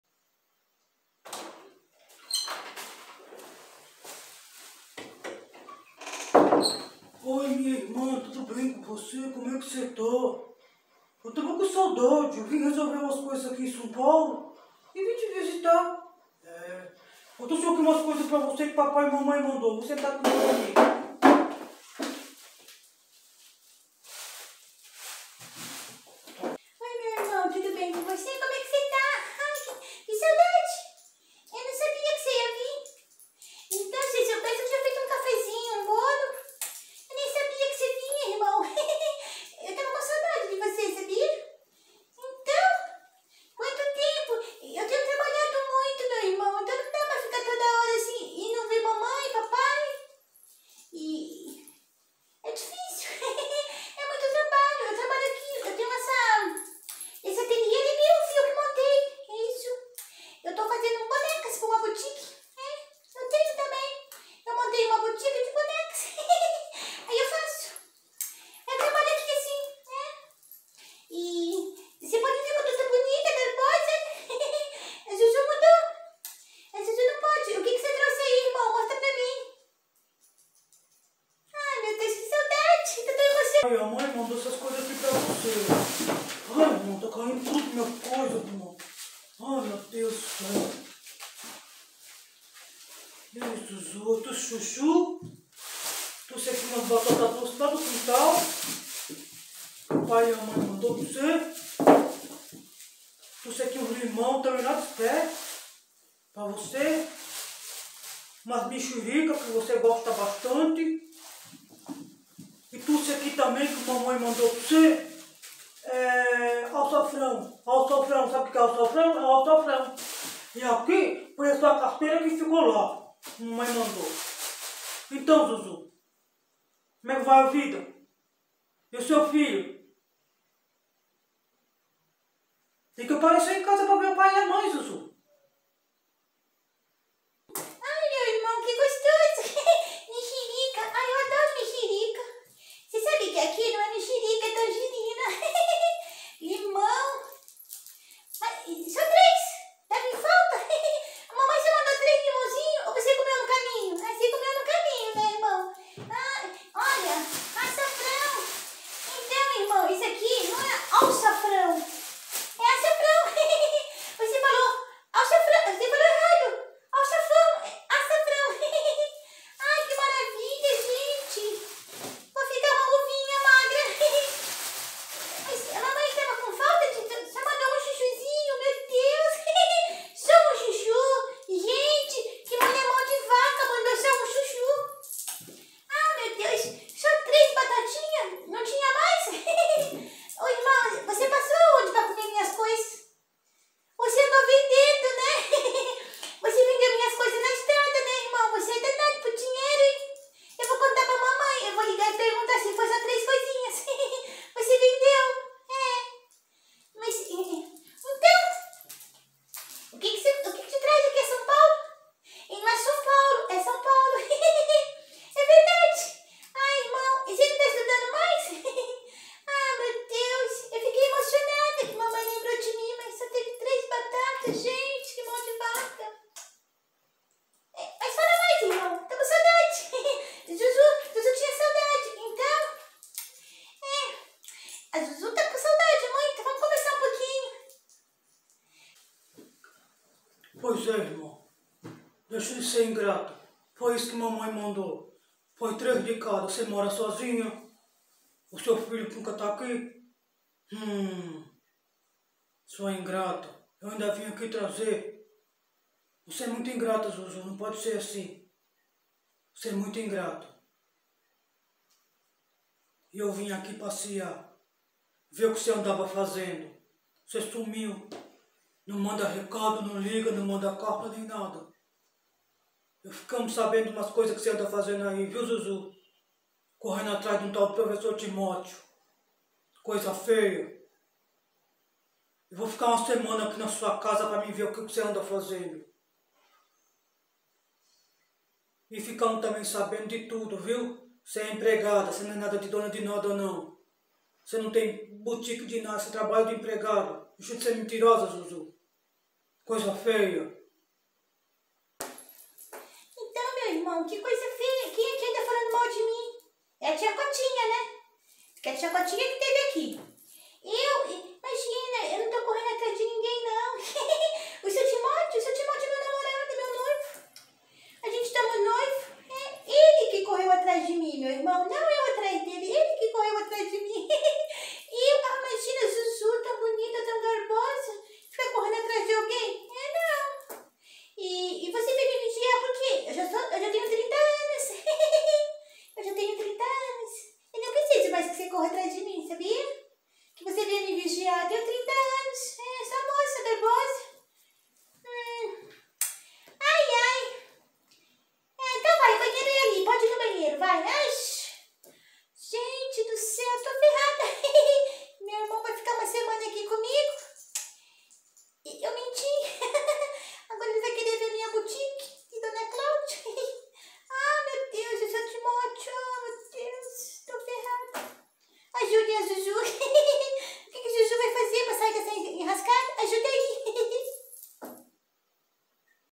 DEU RUIM PRA ZUZÚ
Olá pessoal tudo bem com vcs, no video de hoje a Zuzu vai se da mal seu irmão veio do sul de Minas e vai saber das mentiras de Zuzu., espero que vcs gostem deixe aquele like e passe para os amigos e comentem